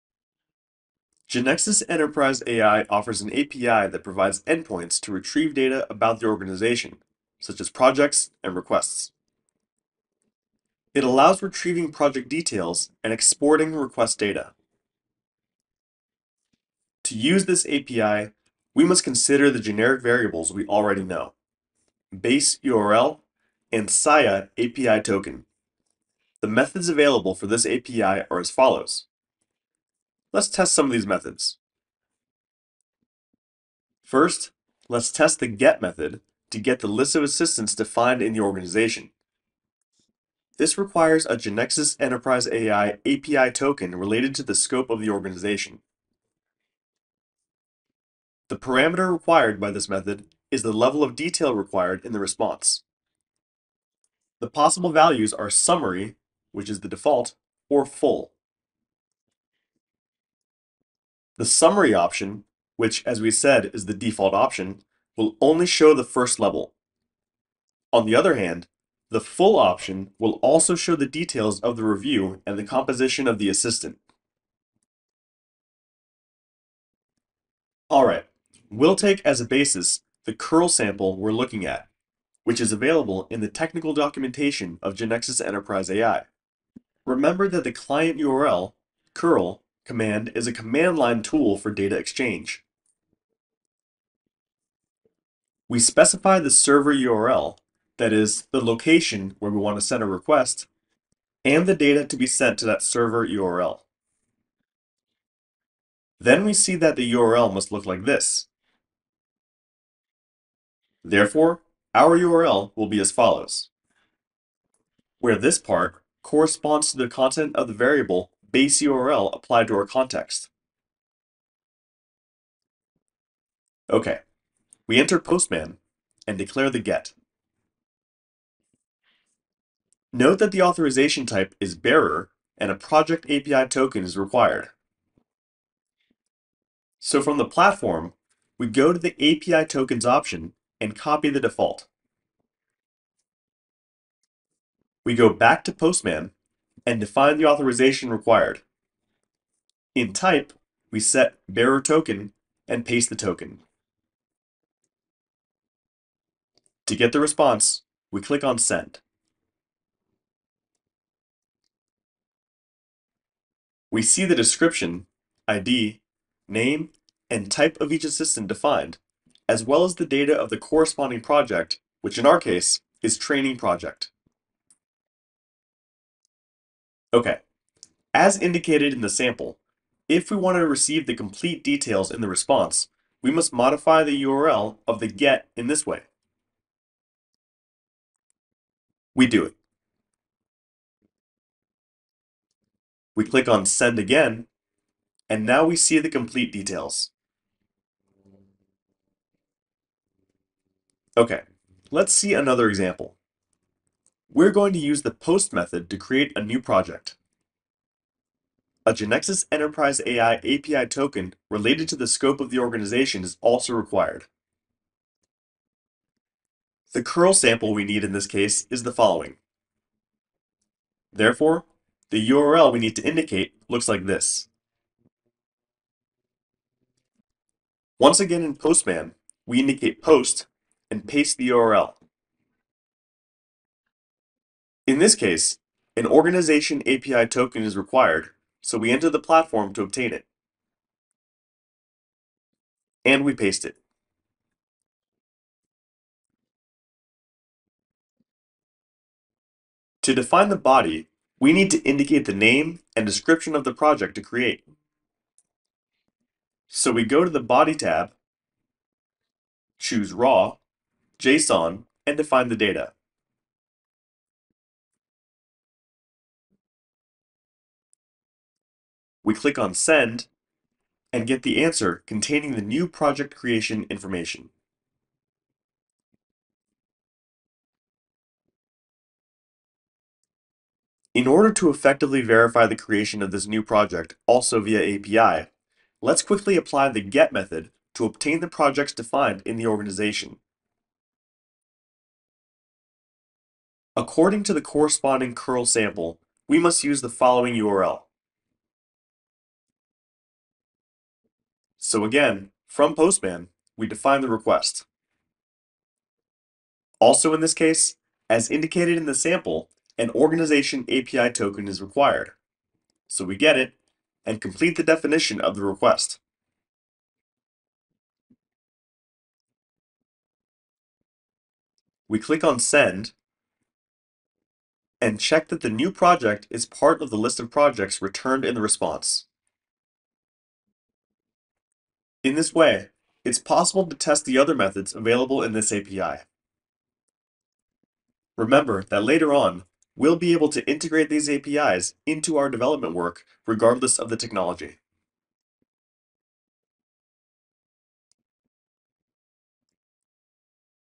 Organization API
GeneXus Enterprise AI offers an API that provides endpoints to retrieve organization data, such as projects and requests.

To understand its use, we see different test examples of the methods available in this API.